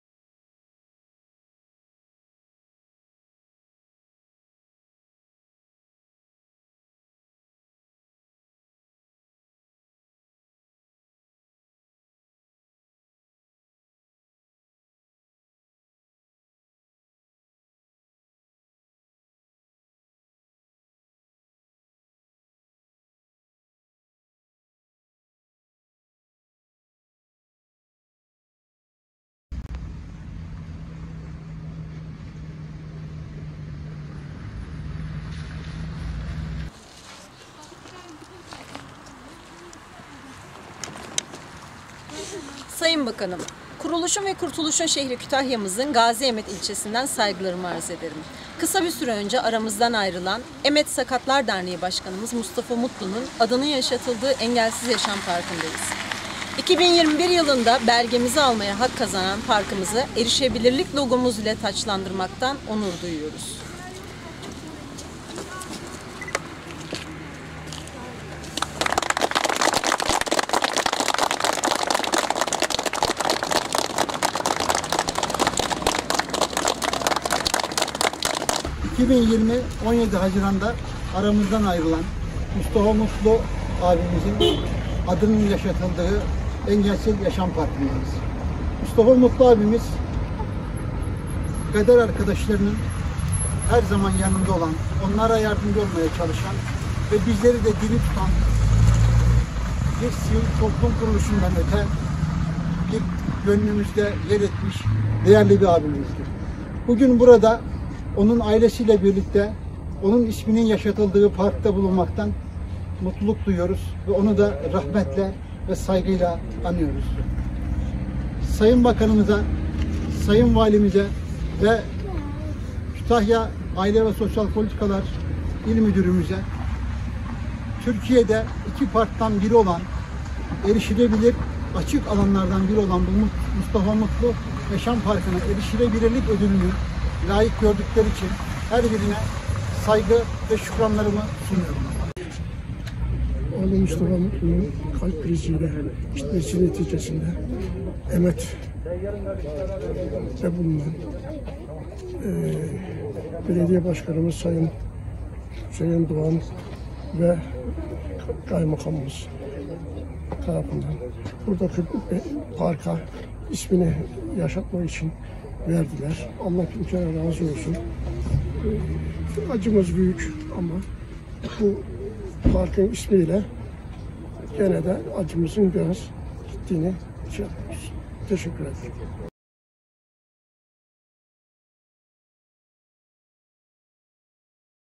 TÜRKİYEDE ERİŞİLEBİLİRLİK BELGESİ ALAN 2 PARK’TAN BİRİ EMET OLDU
Aile ve Sosyal politikalar bakanlığınca 2021 yılında Kütahya’da  Erişilebilirlik belgesi almaya hak  kazanan 20 kurumdan birisi  olan Emet Belediyesi Mustafa Mutlu Cevizdere Engesiz yaşam parkı’nın erişilebilirlik logosu  parkta yerini aldı.
Aile ve Sosyal hizmetler il müdürü Fatma Uz,Emet belediye başkanı Hüseyin Doğan ve parkın isminin verildiği merhum Mustafa Mutlu’nun ailesi, Engelsiz yaşam parkında erişilebilirlik logo takdim töreninde bir araya geldi.
 Aile ve Sosyal hizmetler il müdürü Fatma Uz, 2021 yılında  erişilebilirlik ödülü almaya hak kazanan Mustafa Mutlu Cevizdere Engelsiz yaşam parkı  Erişilebilirlik levhasını vermekten mutluluk duyduğunu ifade etti.
 Törende konuşan Emet belediye başkanı Hüseyin Doğan 17 Haziran 2020 yılında vefat eden Emet sakatlar derneği başkanı Mustafa Mutlu’nun isminin yaşatılması  adına belediye meclisince karar alındığını ifade ederek;
 “ 17 Haziran 2020 ‘de kalp krizinden hayatını kaybeden Mustafa Mutlu abimiz kader arkadaşlarının mutluluğu  ve huzuru için bu tür çalışmalara her zaman öncülük eden bir abimizdi. Bir sivil toplum örgütü başkanlığından öte gönlümüzde yer etmiş bir büyüğümüzdü.Bugün burada onun ailesi ile birlikte isminin bulunduğu parkta bulunmaktayız.Türkiye’De erişilebilirlik belgesi verilen 2 parktan biri olan Mustafa Mutlu Cevzidere Engelsiz yaşam parkımız için ,Sayın bakanımıza,sayın Valimize ve Aile ve sosyal hizmetler il müdürlüğümüze teşekkürlerimizi sunuyoruz.”dedi.
  Erken yaşta oğullarını kaybetmenin üzüntüsünü yaşadıklarını,ancak  isminin  böyle bir parkta yaşatılmasından büyük onur duyduklarını ifade eden baba  Ali Mutlu emeği geien tüm yetkililere  ailesi adına şükranlarını sundu.
 Belediye başkanı Hüseyin Doğan tarafından parkta bulunan çocuklara çeşitli hediye ve ikramlarda bulunuldu.
. Kütahya Valiliği, Kütahya İli Erişilebilirlik İzleme ve Denetleme Komisyonu Tarafından Uygun Görülen kişi ve kurumlara   Erişilebilirlik ödülleri 3 Aralık 2021 de verilmişti.Emet belediyesine ; Kütahya Valisi  Ali Çelik Tarafından, erişilebilirlik ödülü; Emet Sakatlar derneği başkanı merhum Mustafa Mutlu’nun adının bir parkta yaşatılması ile farkındalık oluşturulması adına takdim edilmişti.
2021 yılında Kütahya genelinde Hezar Dinari Kültür Merkezi, Sağlık Bilimleri Üniversitesi Ağız ve Diş Sağlığı Merkezi ile Sağlık Bilimleri Üniversitesi Tavşanlı Meslek Yüksek Okulu, İl Emniyet Müdürlüğü, Pazarlar İlçe Emniyet Amirliği ve Şehit Mehmet Kartal Polis Merkezi Amirliği, Simav Kışla Caddesi, Simav Eski Garaj Caddesi ve Simav 4 Eylül Açık Otoparkı, Emet Mustafa Mutlu Engelsiz Yaşam Parkı, Altıntaş Yürüyüş Yolu, Aslanapa Kültür Evi, Şehit Mustafa Baş Aile Sağlığı Merkezi, 100. Yıl Aile Sağlığı Merkezi ve Şaphane Toplum Sağlığı Merkezi, Kütahya İl Milli Eğitim Müdürlüğü, Domaniç Hisar İlkokulu ve Borsa İstanbul Mesleki Teknik Anadolu Lisesi, Gediz Kız Yurt Müdürlüğü Binası ve Gediz Erkek Yurt Binasının erişilebilirlik ödül ve belgeleri verildi.